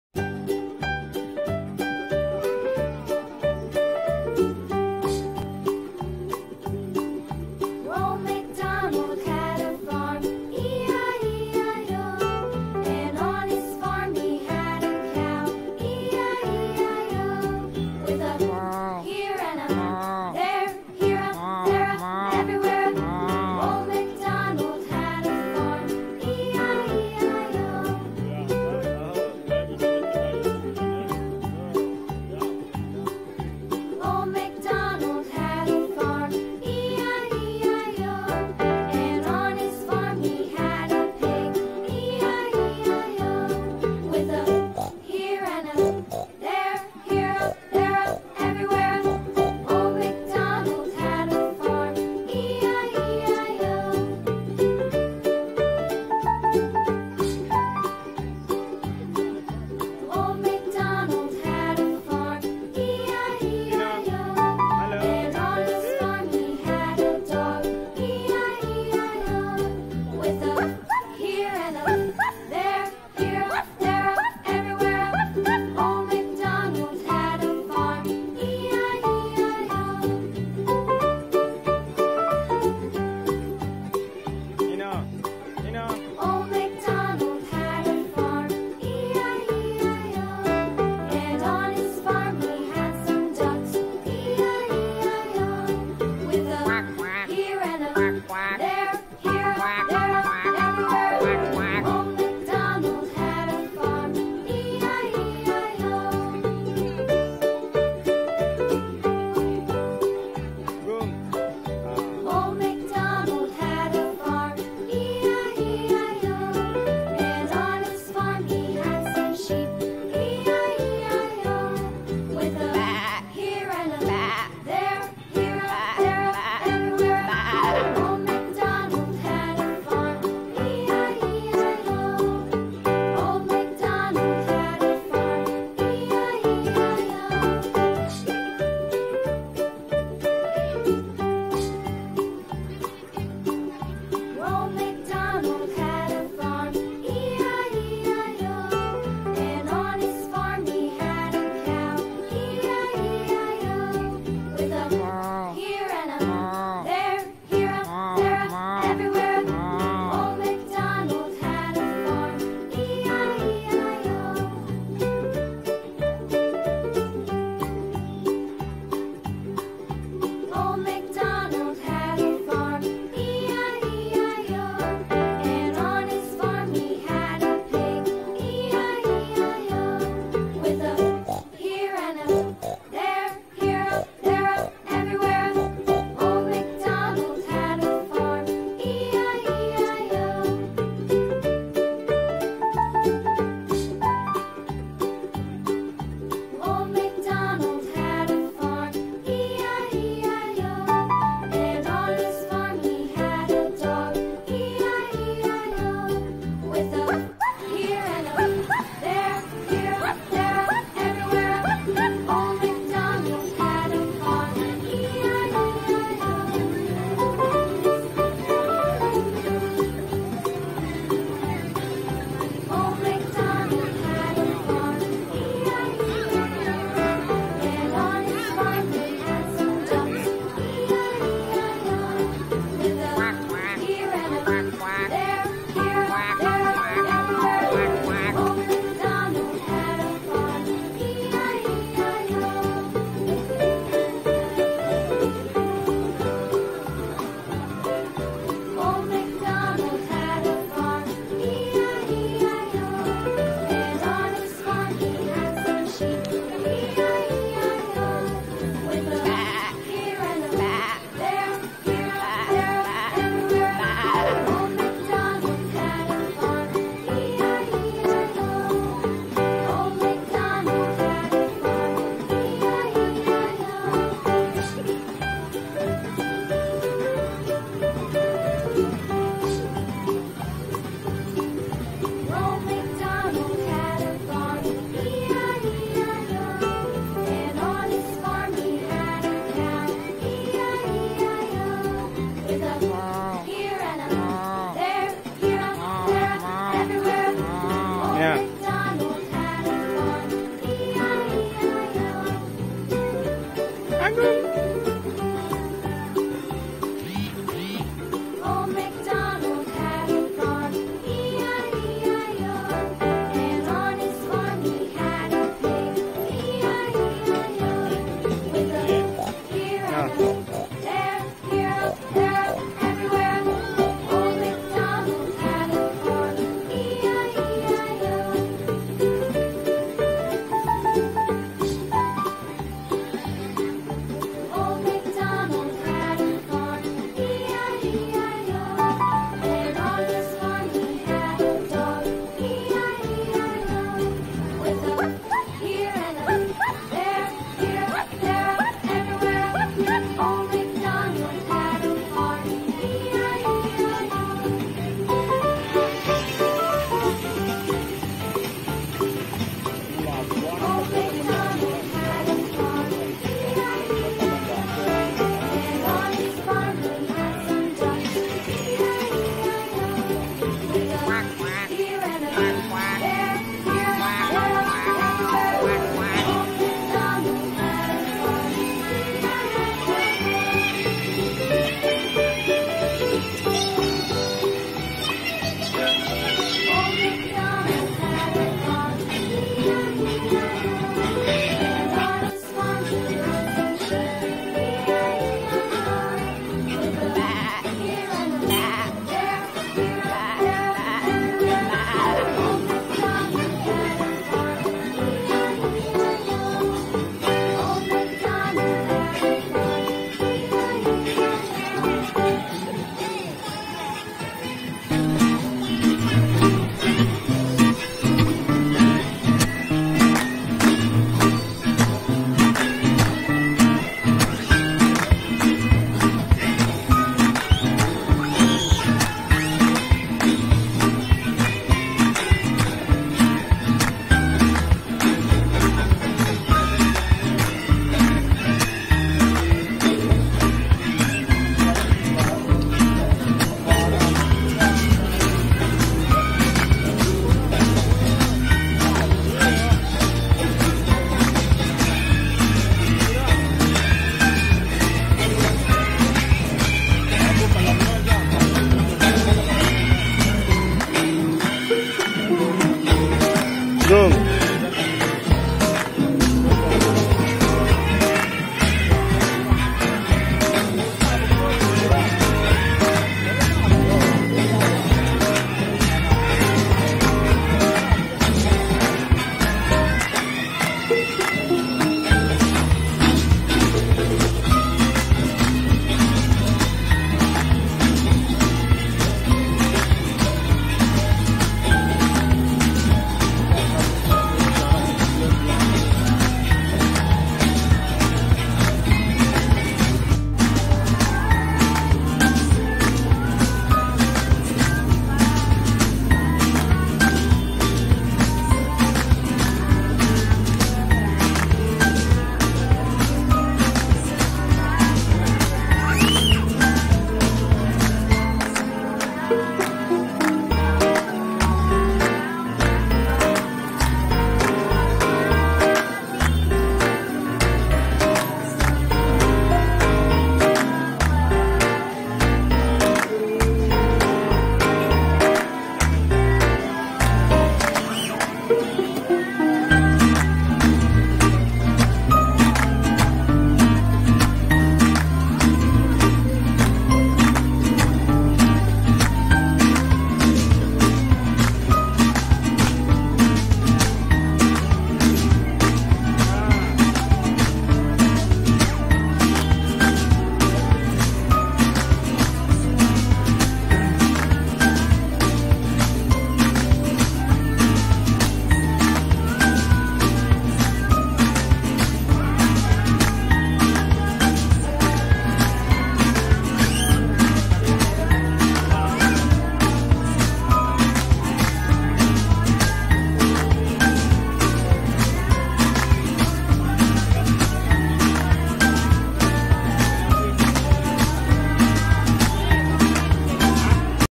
Arvino Naik Odong-odong Lucu
arvino naik odong-odong lucu di malam hari dekat rumah.